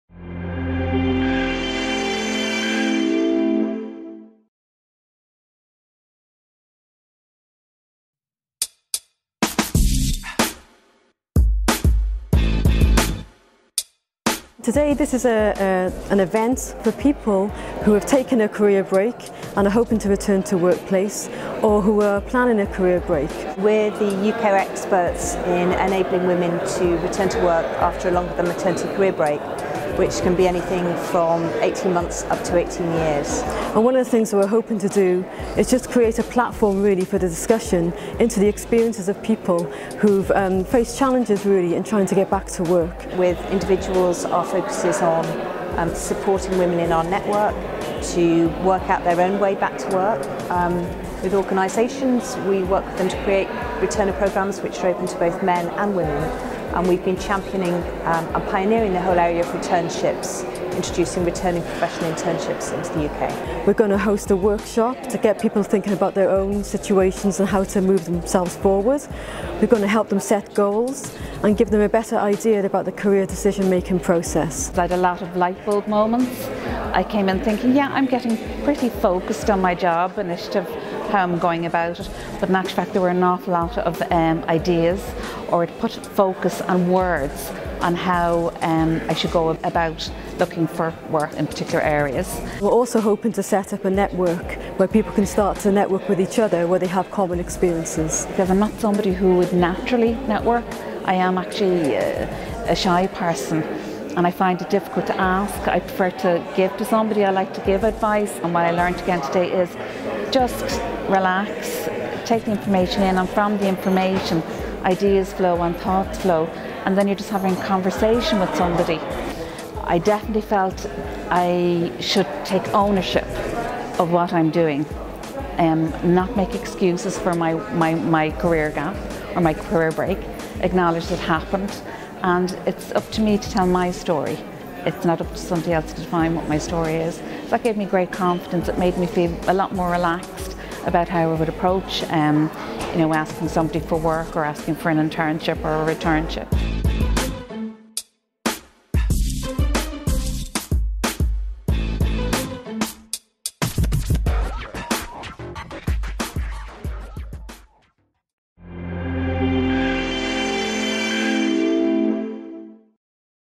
Trinity Returners
An overview of the inaugural Trinity career returners event. Set up of a network for graduates looking to access meaningful resources when trying to return to work after a  significant career break for family or other reasons.

Speakers:
Julianne Miles:Co-Founder & Director, Women Returners
Carolanne Mc Partlan: Event participant
Liz Cleves: Event organiser